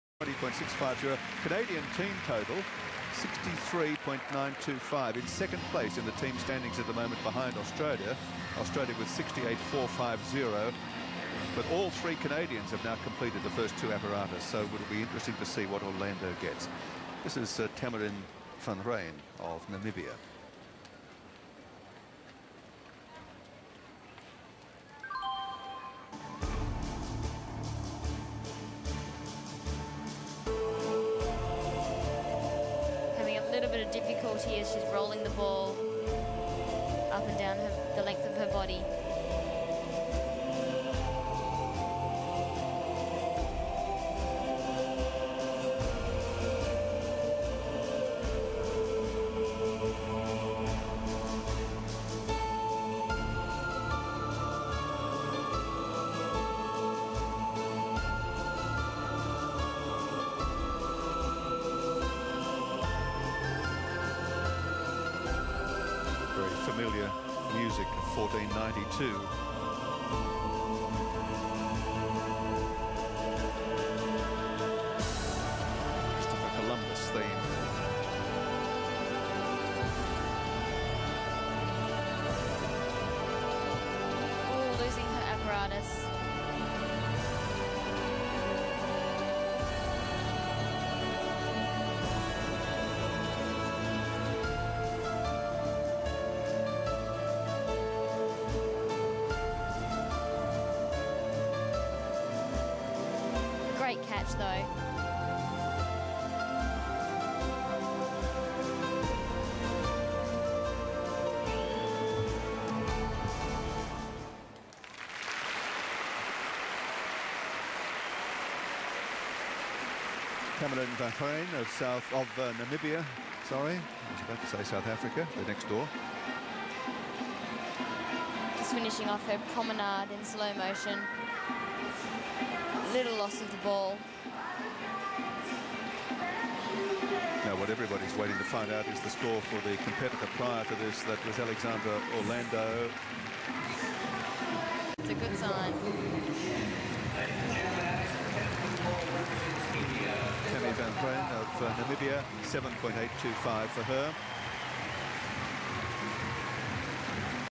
Tamarin-Jade Van Rhyn 2006 Commonwealth Games Ball Team Final
Team Final Ball routine of Tamarin-Jade Van Rhyn from Namibia in the 2006 Commonwealth Games.